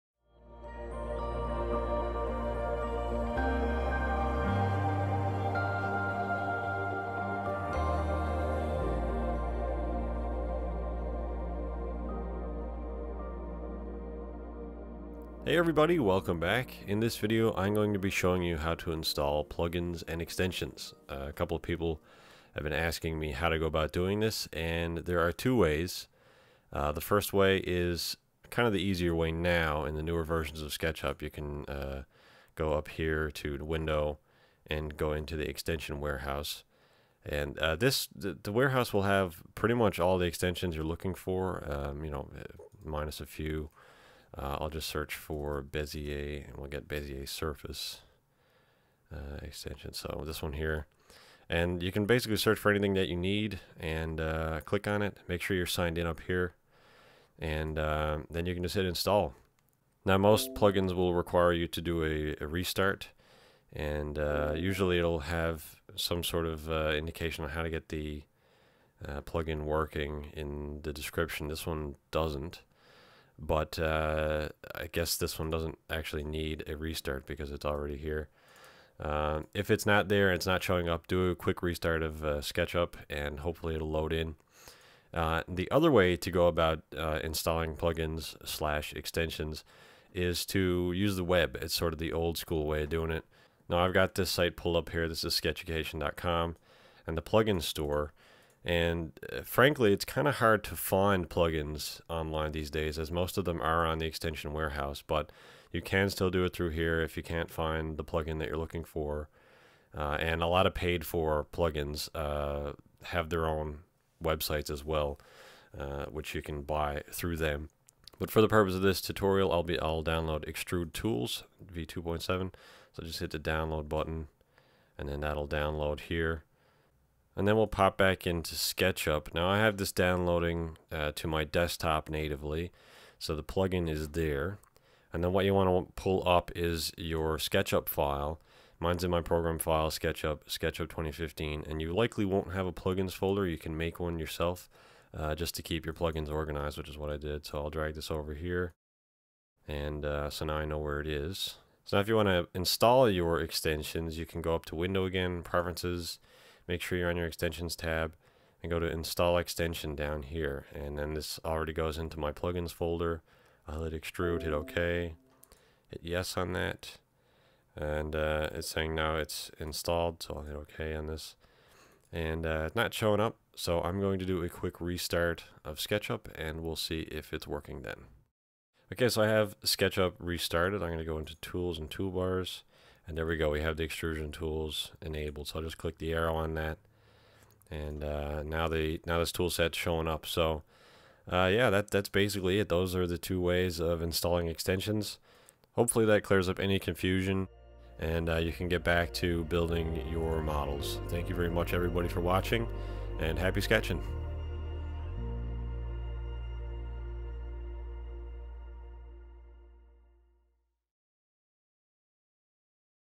SketchUp 2015 Lessons: Installing Plugins & Extensions
This week I show you how to install plugins the old school way, and through the Extension Warehouse.

Also follow me on:
Facebook.com/aaronbishopdesign
Twitter.com/aaronbdesign
Behance.net/aaronbishopdesign
Instagram.com/aaronbishopdesign